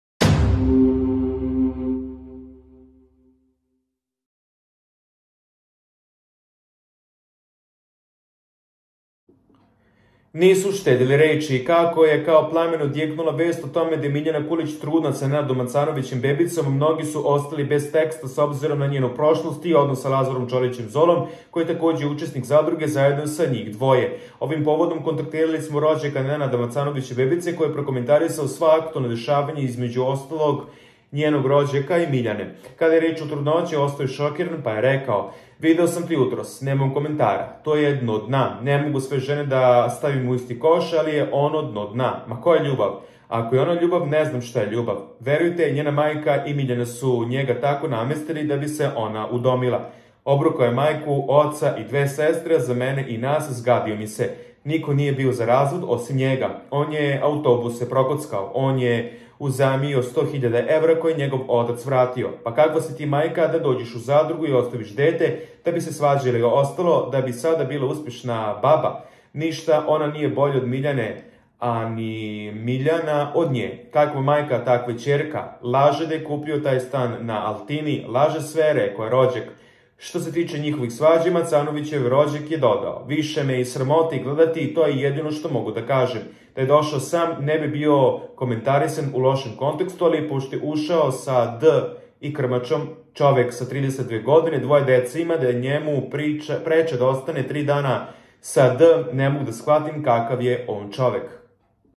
SKANDALCINA: HITNO oglasavanje porodice Nenada Macanovica nakon Miljanine trudnoce! SOK
Nisu štedeli reči.